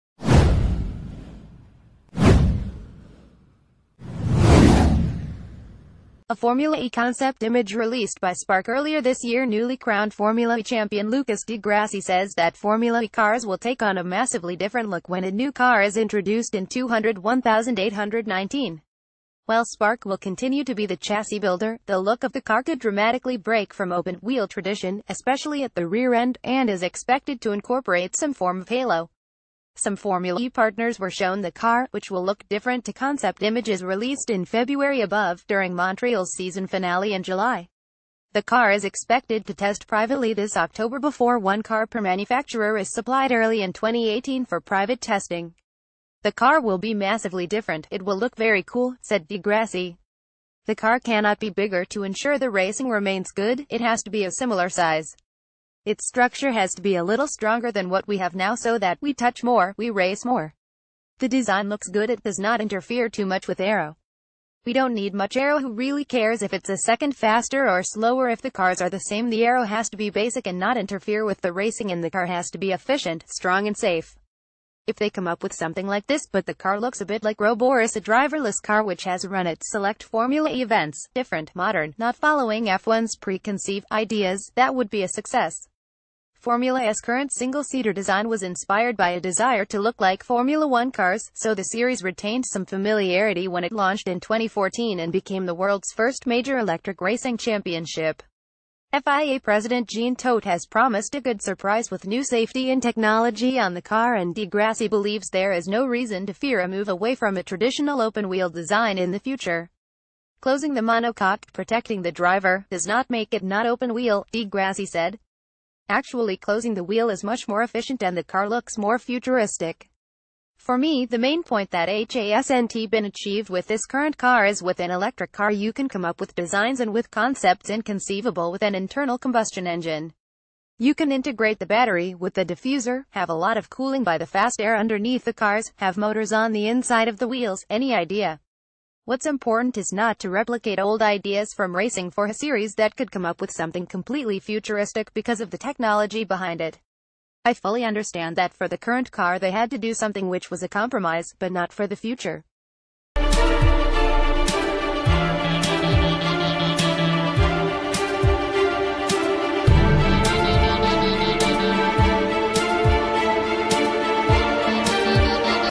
Breaking News | Di grassi: new fe cars to look ‘massively different’
Di grassi: new fe cars to look ‘massively different’

A Formula E concept image released by Spark earlier this year Newly crowned Formula E champion Lucas...